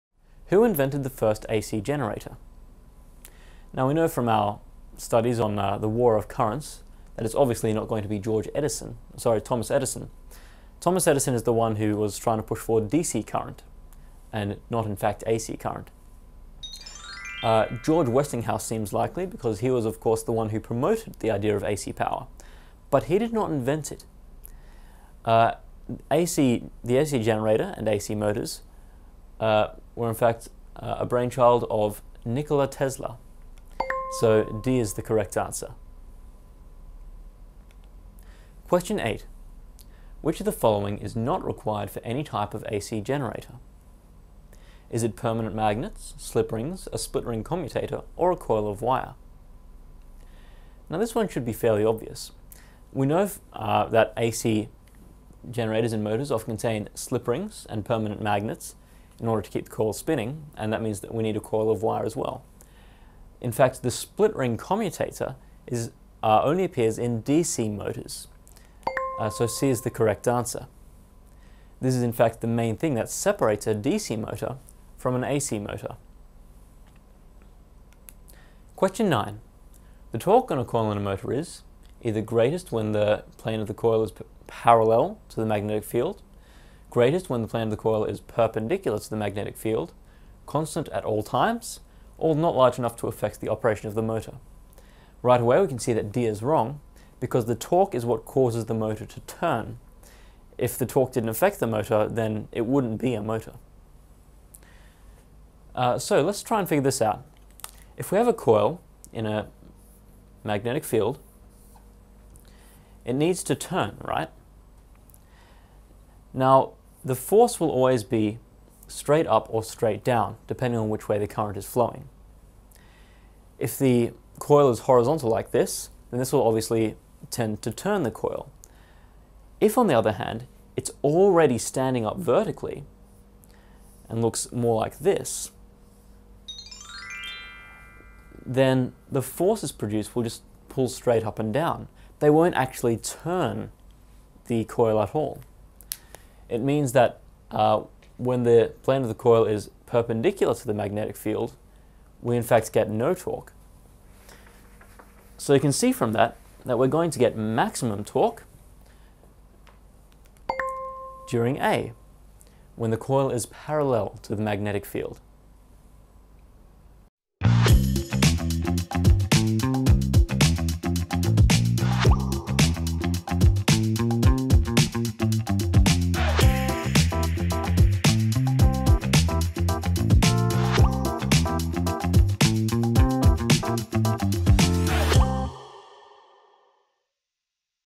💯 √ Nikola Tesla | Spring-Ring Commutator | Torque on Coil in Motor
Question 7

Who invented the first AC generator?

(A) George Westinghouse
(B) Thomas Edison
(D) Nikola Tesla

Answer: (D)

Question 8

Which of the following is not required for any type of AC generator?

(A) permanent magnets
(B) slip rings
(D) a coil of wire

Question 9

The torque on a coil in a motor is:

(A) greatest when the plane of the coil is parallel to the magnetic field.
(B) greatest when the plane of the coil is perpendicular to the magnetic field.
(D) not large enough to affect the operation of the motor.

Answer: (A)